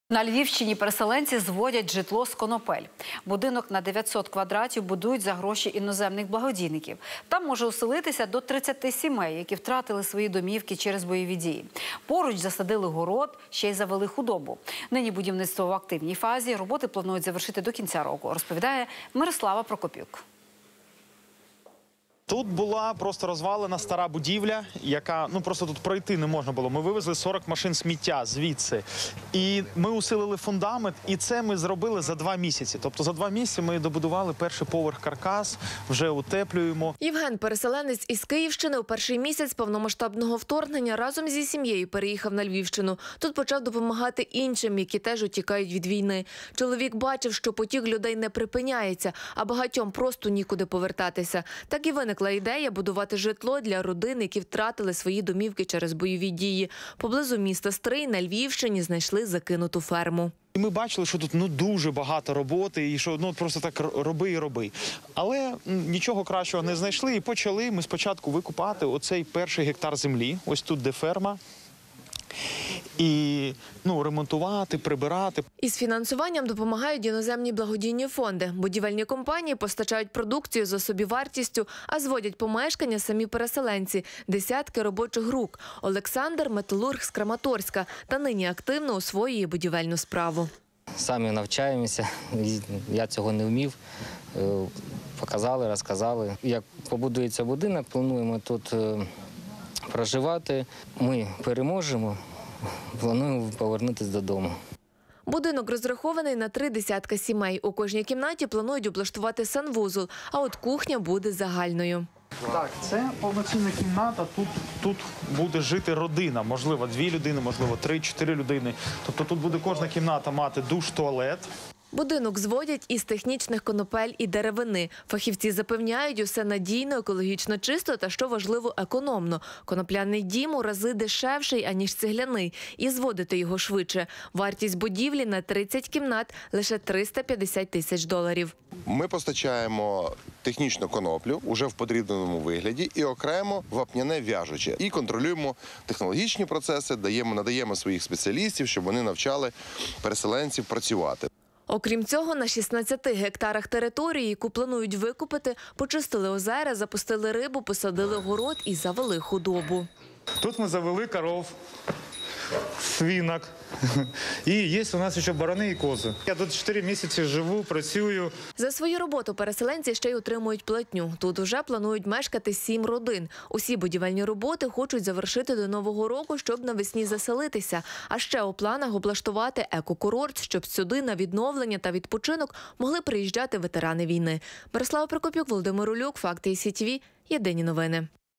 Дім із КОНОПЛІ! На Львівщині будують будинки, в яких мріють жити ВСІ
На Львівщині переселенці зводять житло з конопель. Будинок на 900 квадратів будують за гроші іноземних благодійників. Там зможе оселитися до 30 сімей, що втратили свої домівки через бойові дії. Поруч засадили город, а ще завели худобу. Нині будівництво в активній фазі, роботи планують завершити до кінця року. Репортаж Мирослави Прокопюк.